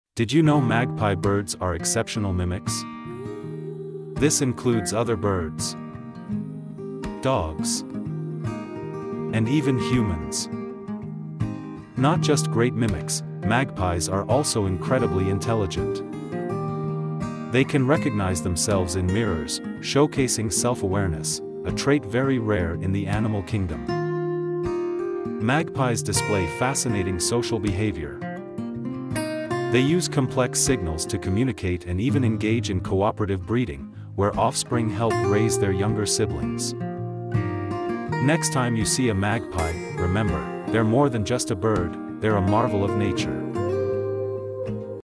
The Chatterbox of the Skies: Amazing Facts About the Magpie Bird's Secret Abilities!
Take flight into the enchanting world of the Magpie bird in this captivating YouTube shorts video. Join us as we uncover intriguing facts about this intelligent and charismatic creature. With mesmerizing visuals, we'll introduce you to the dazzling plumage and captivating behaviors that make Magpies stand out in the avian kingdom.

Discover their remarkable problem-solving skills, their fascinating social dynamics, and the unique vocalizations that echo through their habitats. Delve into the cultural symbolism and folklore surrounding the Magpies, and the curious myths that have been woven around them.

Brace yourself for an educational and visually stunning exploration of facts about the Magpie bird that celebrates the brilliance and wonder of nature's shiny avian jewel.

Watch now and be amazed by the beauty and intelligence of the Magpie!